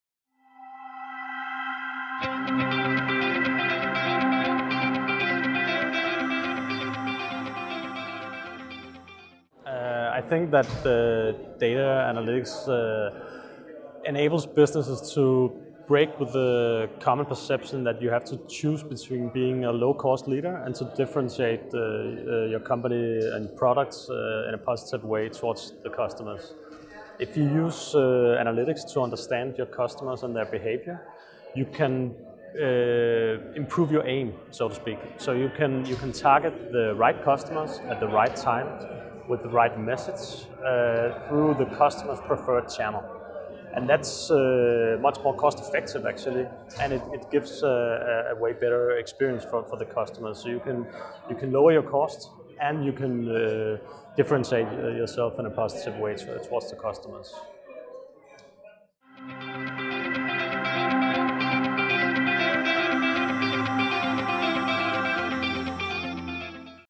Kasper Koluda Andersen: How to best use data as a competitive power?
During the Business Analytics in København, Denmark, Kasper Koluda Andersen shares his thoughts on how to best use data as a
competitive power.

Icebreaking - Matchmaking - Smilemaking

Austria | Denmark | Finland | Germany | The Netherlands | Norway | Singapore | Sweden | Switzerland | Turkey I Pan-European I DACH I Nordic